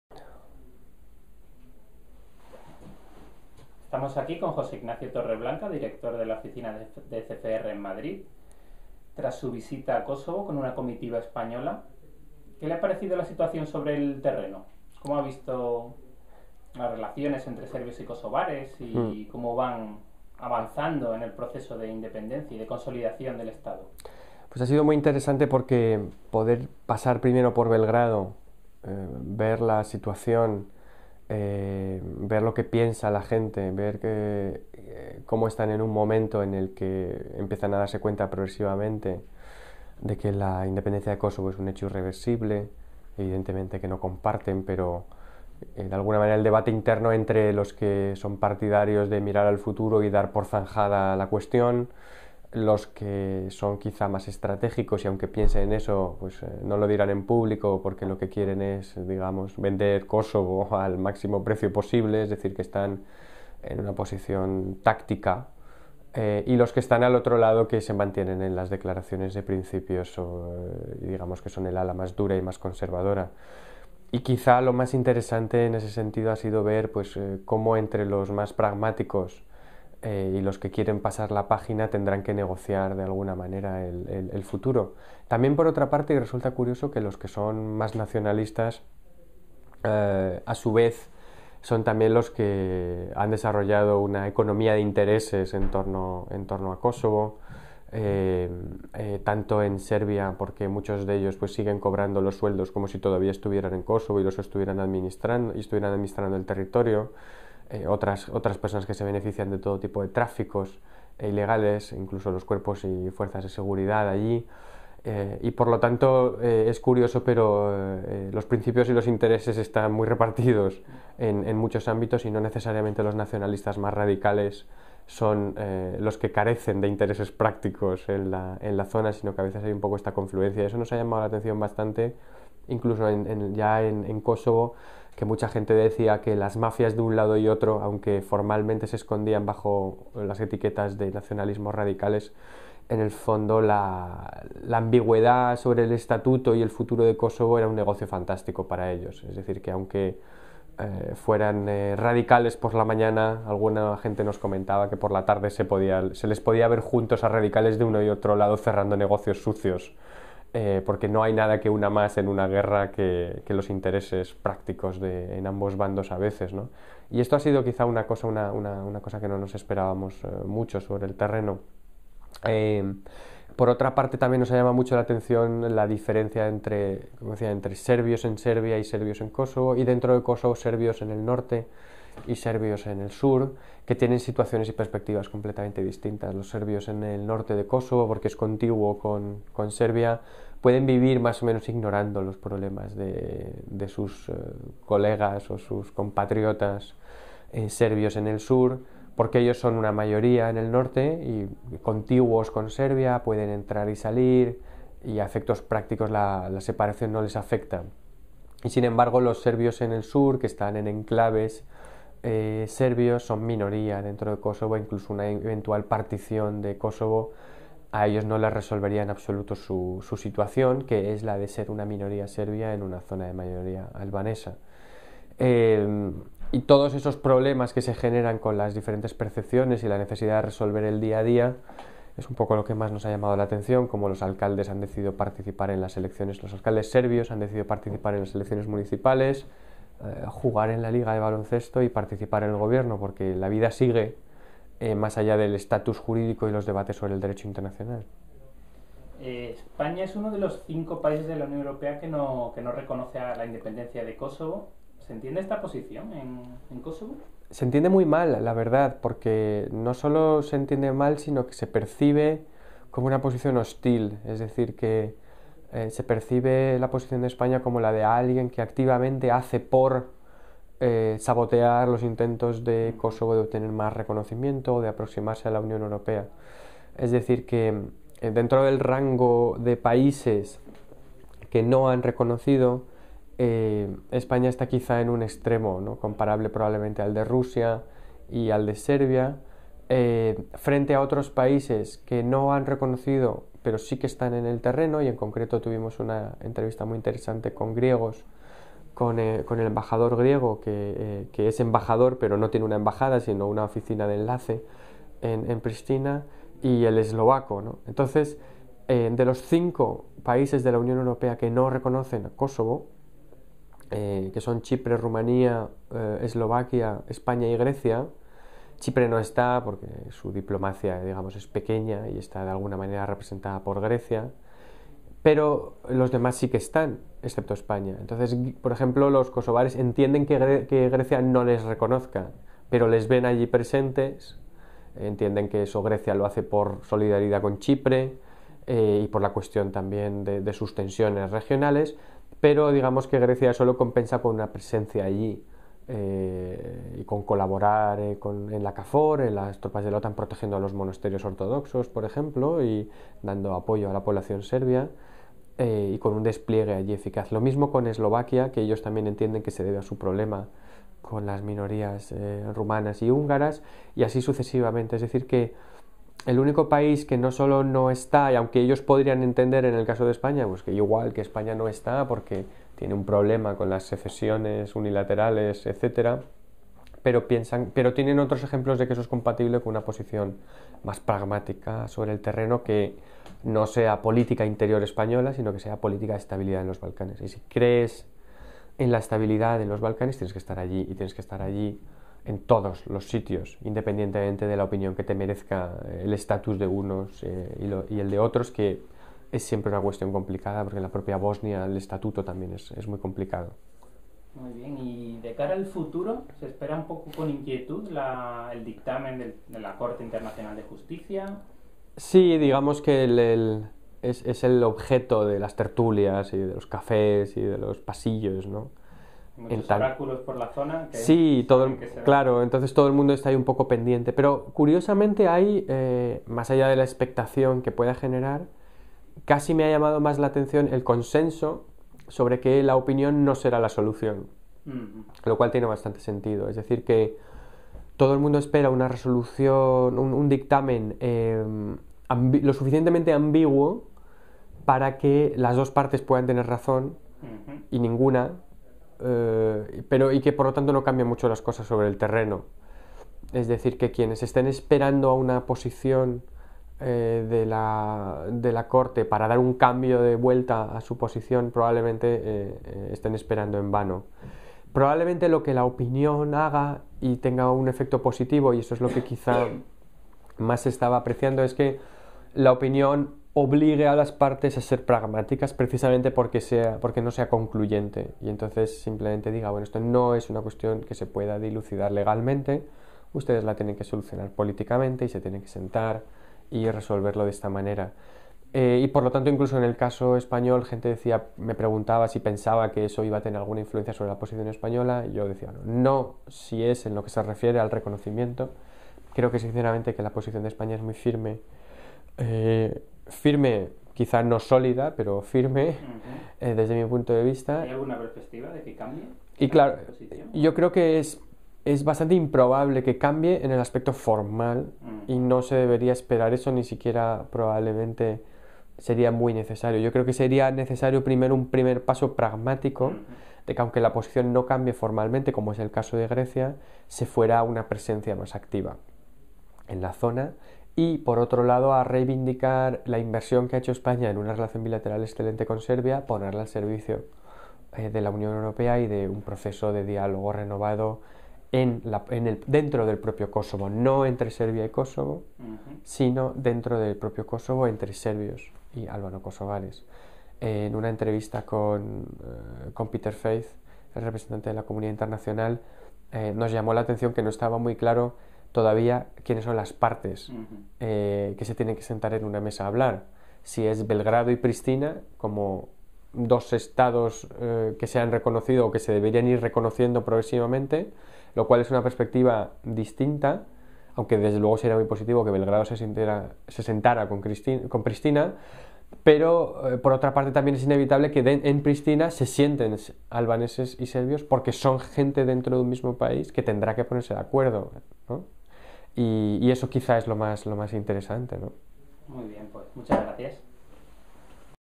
José Ignacio Torreblanca habla sobre Kosovo
José Ignacio Torreblanca, director de la oficina de ECFR en Madrid, habla sobre España y la independencia de Kosovo tras su visita a la zona.